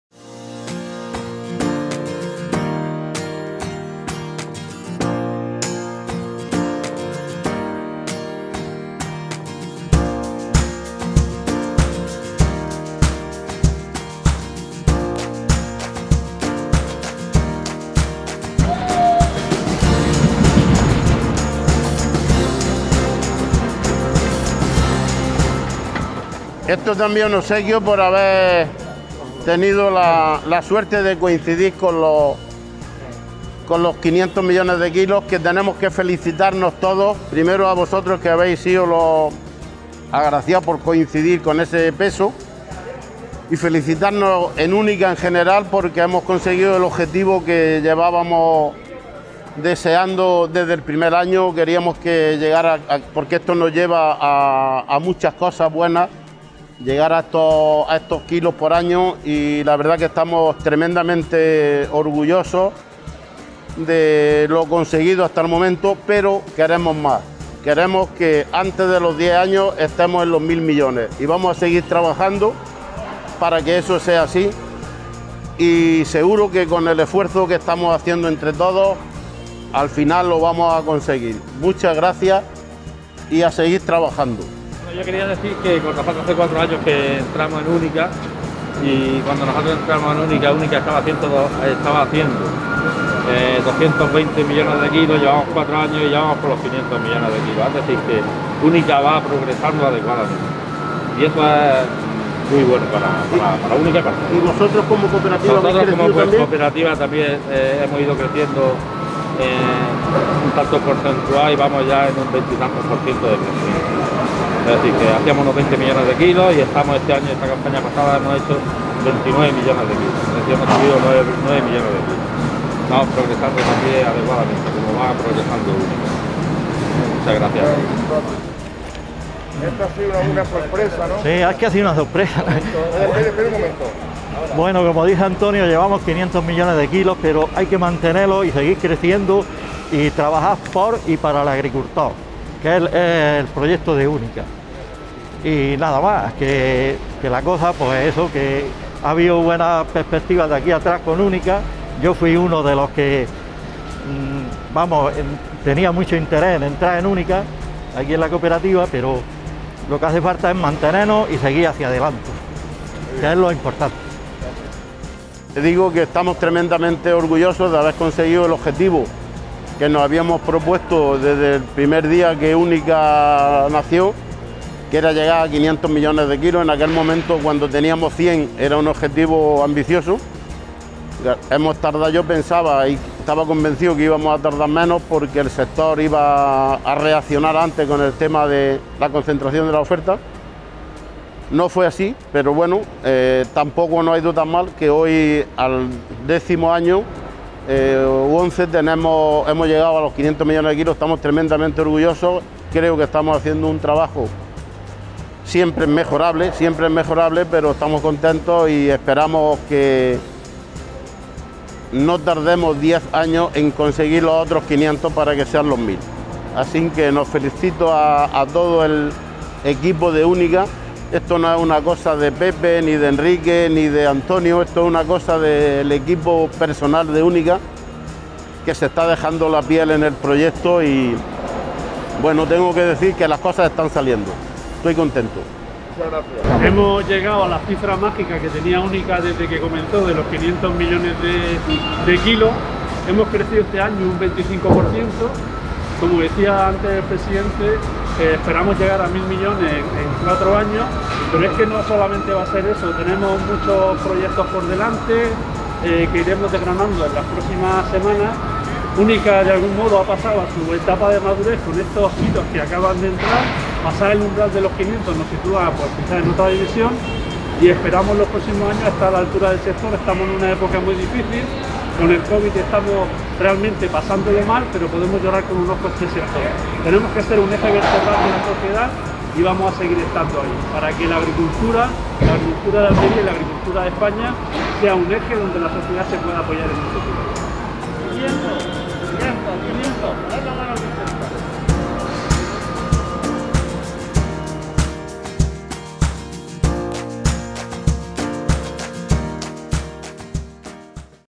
UNICA alcanza los 500 millones de kilos en una sola campaña
Unica Group ha alcanzado la cifra récord de los 500 millones de kilos al cierre de la campaña 2019/20, lo que supone un incremento del 25% respecto a la campaña anterior y representa para la cooperativa una cifra histórica alcanzada en una sola campaña.